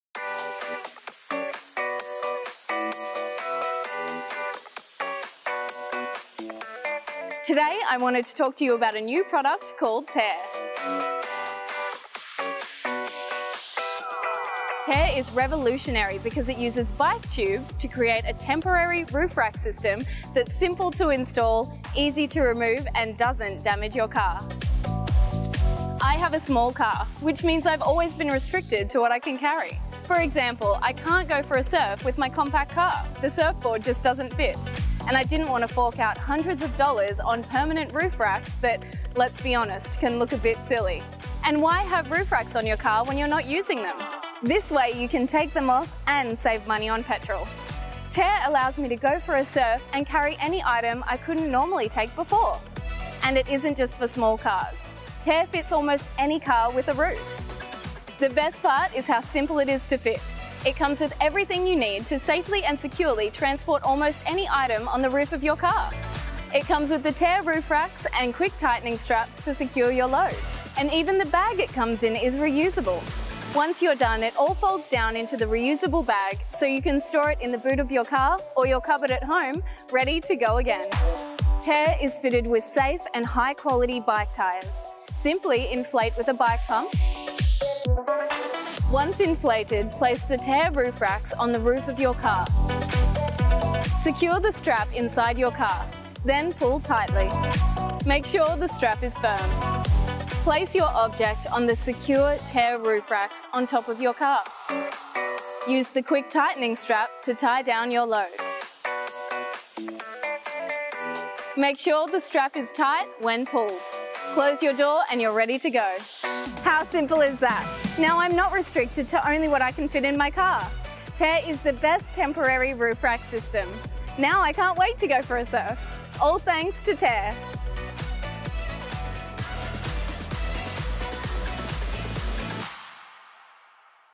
ULTIX TAIR: Revolutionary Inflatable Car Roof Rack
Ultix Tair Rack is an innovative inflatable car roof rack made of bike inner tube. It allows you to transport any sports equipment and bulky cargo easily on any vehicle. It takes just 5 minutes or less to install Tair Rack and it is entirely portable. All you need is to fold it up to a shoebox-size and store in the trunk of your car.